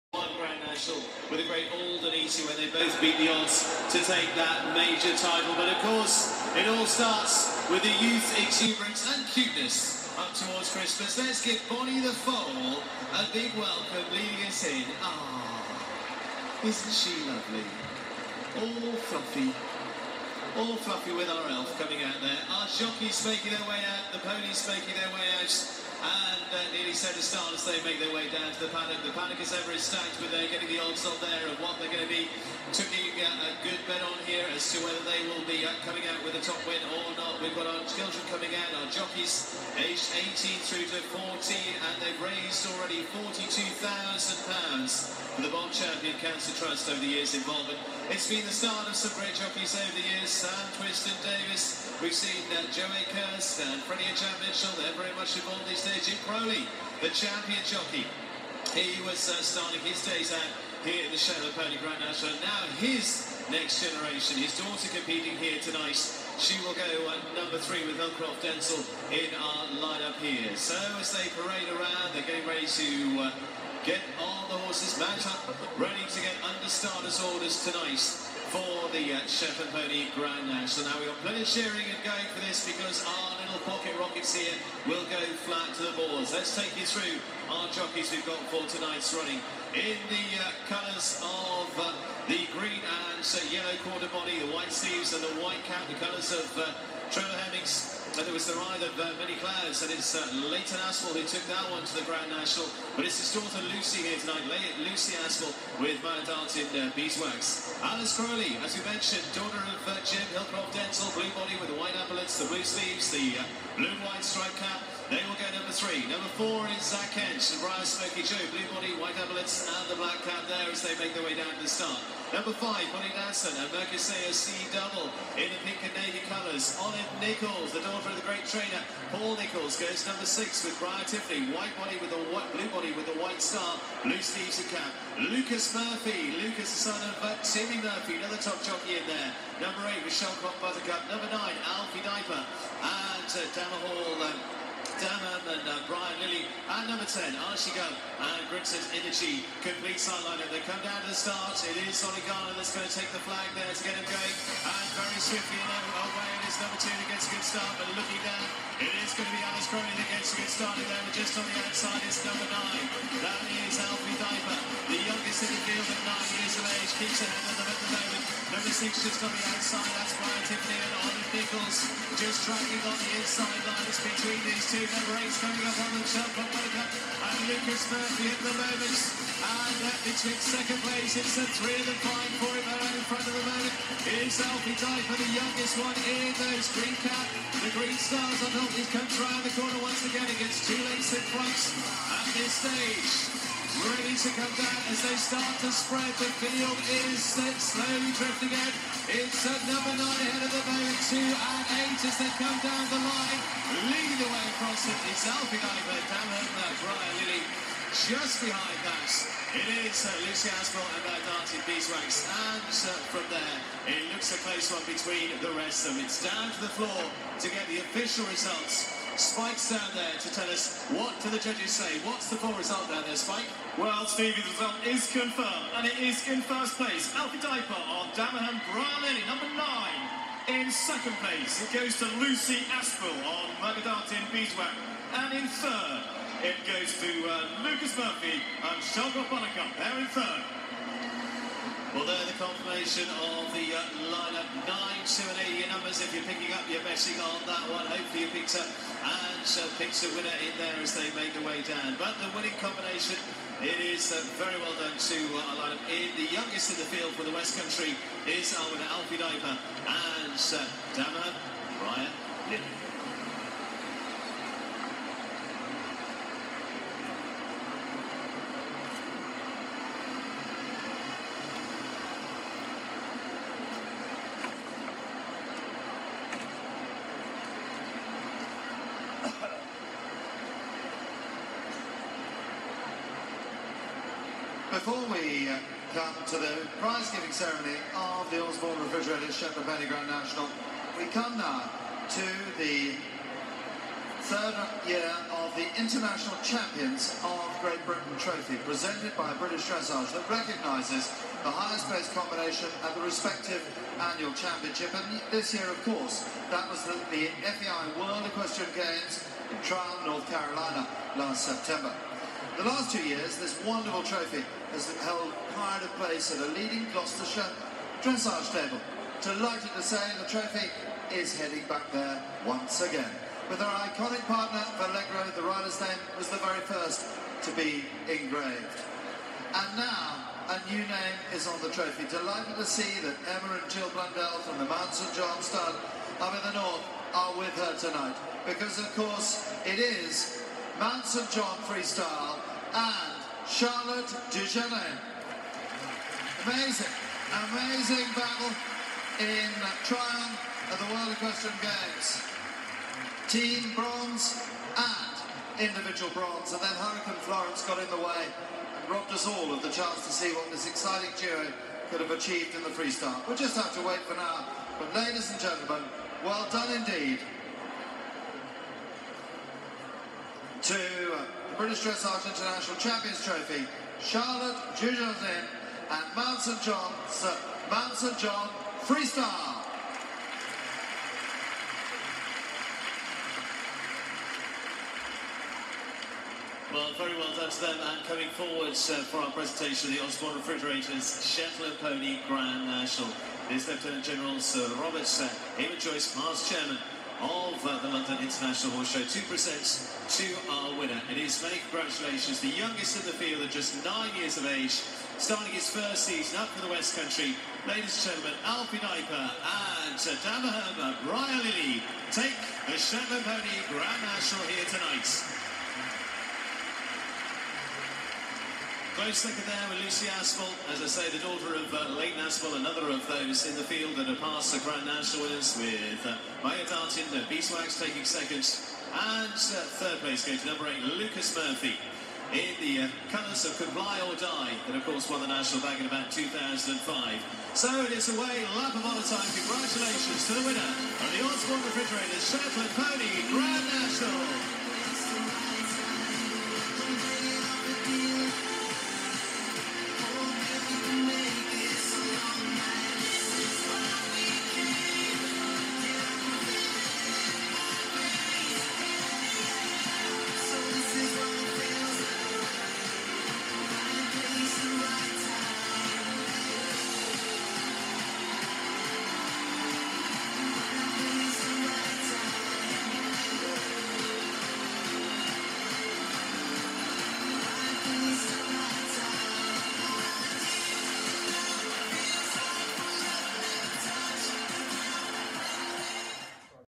2018 Olympia Shetland Grand National. Our very own DAMERHAM BRIAR LILLY with Alfie Diaper.
Winner Winner Winner Tuesday 18th December 2018